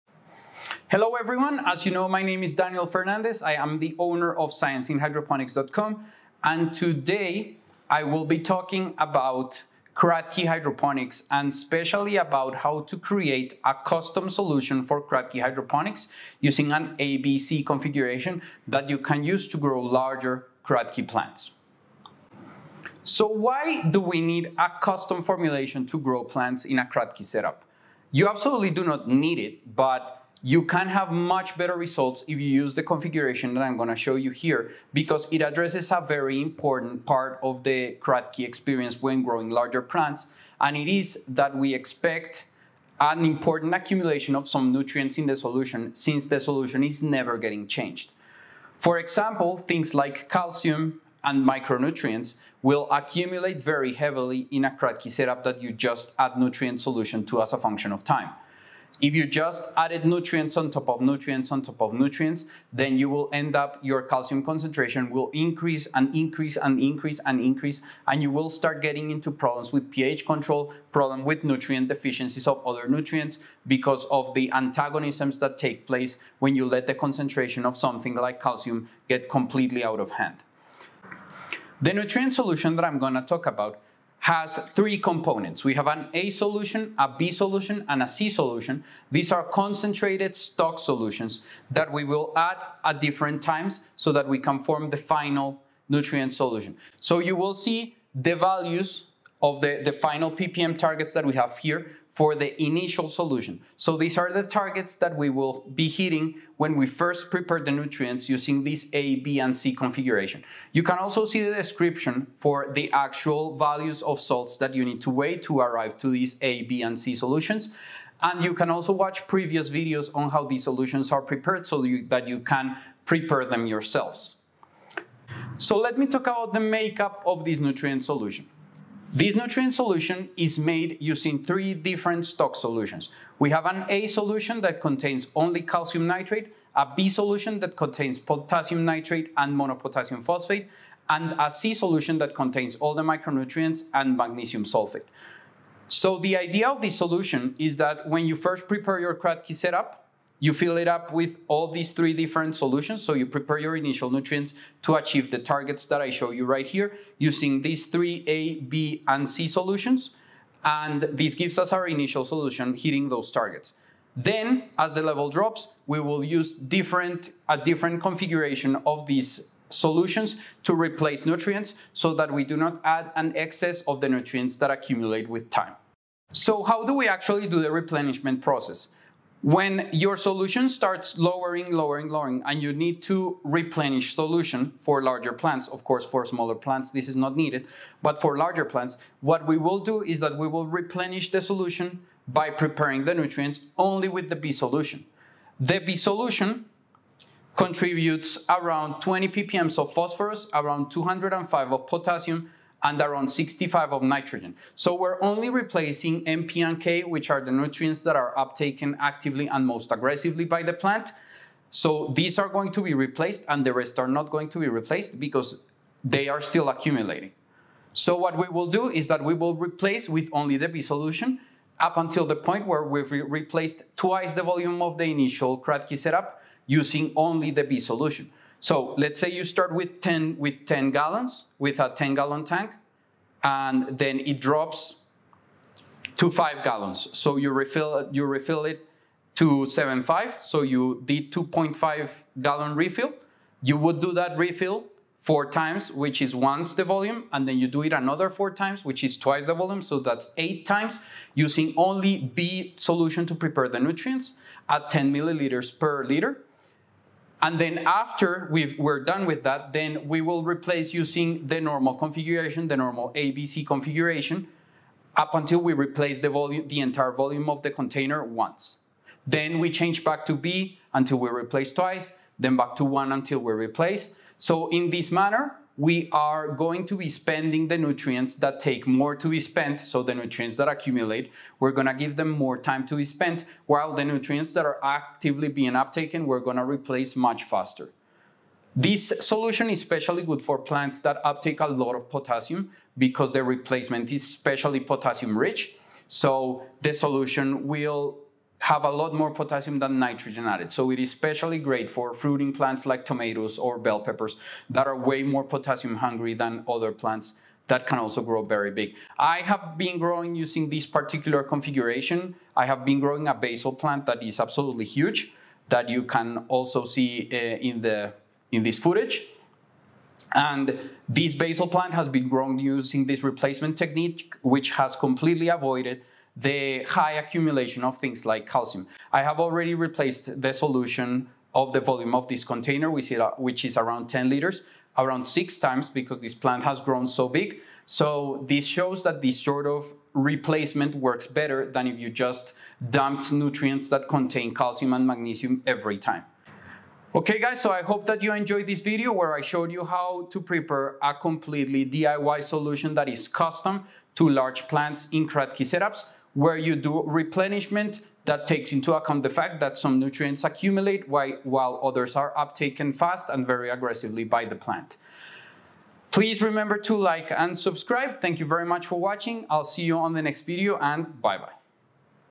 How to DIY specific nutrients for Kratky
In this video, I show you how to prepare an A/B/C nutrient combination for your Kratky setup. This allows you to replenish your Kratky system while minimizing the risk of toxic nutrient accumulation due to excessive addition of Calcium, Magnesium, or micronutrients.

The following quantities are for the preparation of 250mL of each solution. Note that the exact salts mentioned should be used. If using salts with different degrees of hydration or chemical composition, adjustments should be made. Monopotassium phosphate is present in both B and C solutions. All amounts are in grams.

A Calcium Nitrate (ag grade)         16.863
B Potassium Nitrate                         11.782
B Monopotassium Phosphate         2.25
C Anhydrous Iron Sulfate                 0.163
C Anhydrous Manganese Sulfate 0.069
C Zinc Sulfate Monohydrate         0.034
C Copper Sulfate Pentahydrate         0.005
C Boric Acid                                 0.072
C Magnesium Sulfate Heptahydrate 12.677
C Sodium Molybdate Dihydrate         0.003
C Disodium EDTA                         0.725
C Monopotassium Phosphate         2.25

The solutions are 1:100. This means that you need to use 10mL A + 10mL B + 10mL C for the preparation of each liter of the initial Kratky solution. When replenishing with B solution, replenish at 10mL/L of added solution. When replenishing with A/B/C, use 10mL/L of each.